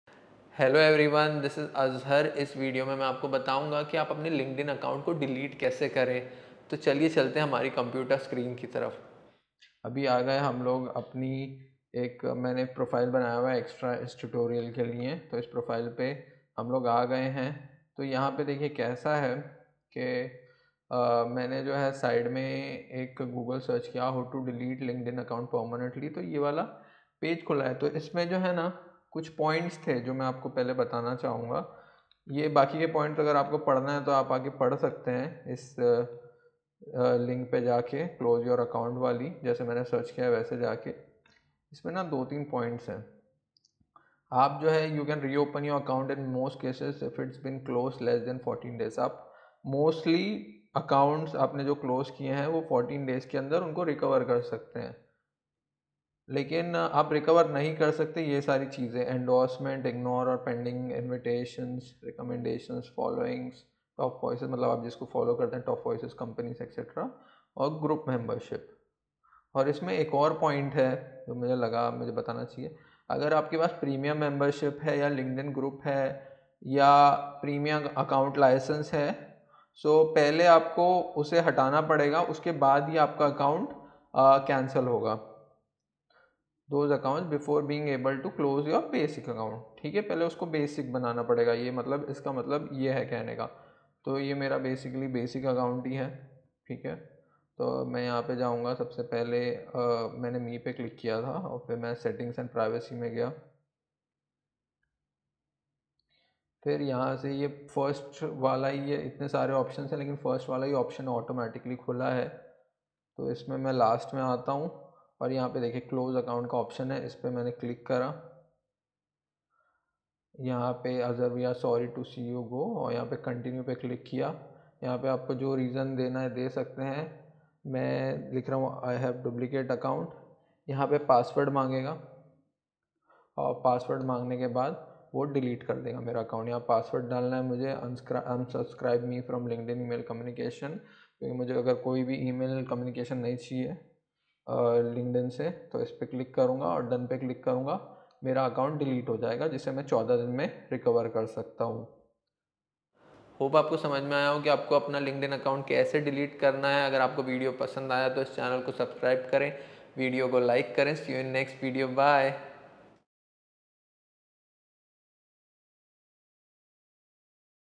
How to delete linkedin account permanently? - Hindi
Is video mein, maine Linkedin Account ko Permanently Delete karne Ka Tareeka Bataya hai.